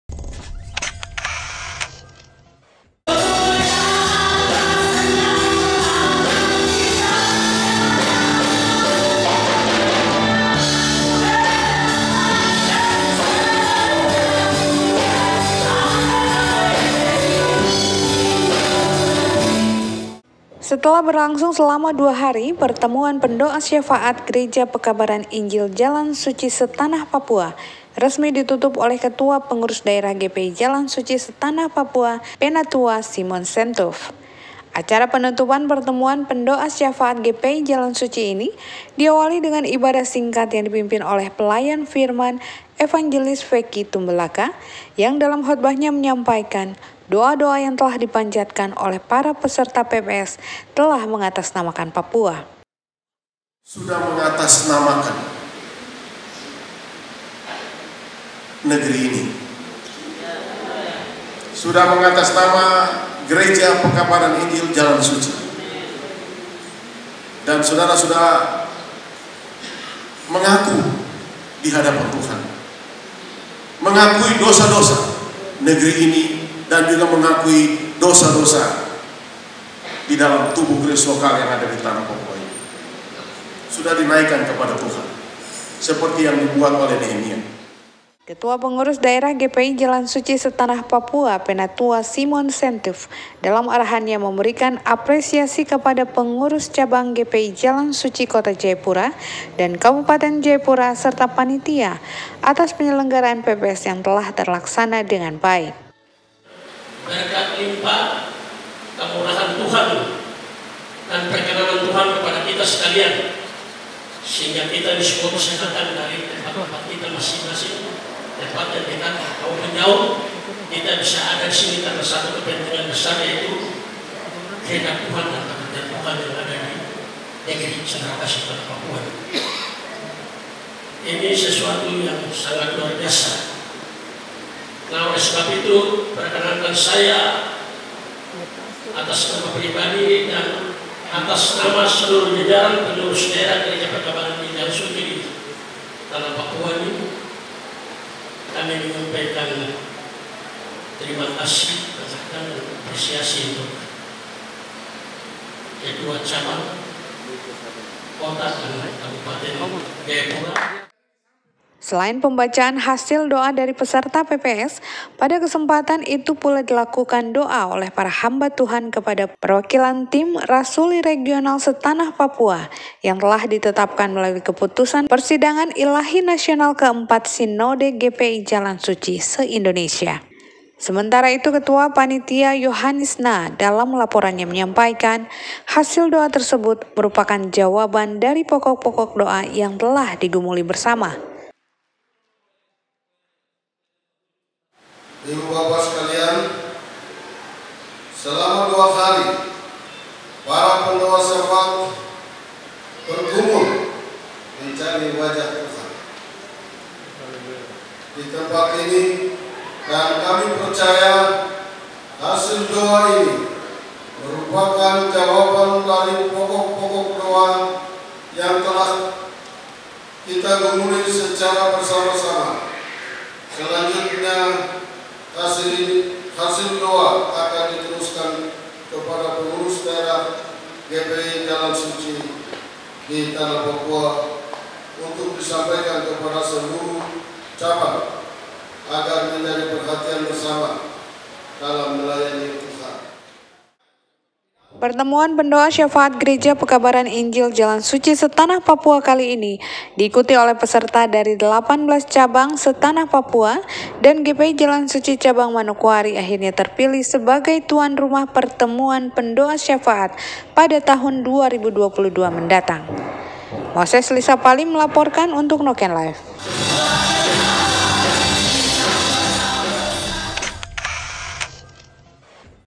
PERTEMUAN PENDOA SYAFAAT GPI SE TANAH PAPUA RESMI DITUTUP
SETELAH DUA HARI BERLANGSUNG, PERTEMUAN PENDOA SYAFAAT GEREJA PEKABARAN INJIL (GPI) "JALAN SUCI" SE-TANAH PAPUAN RESMI DITUTUP.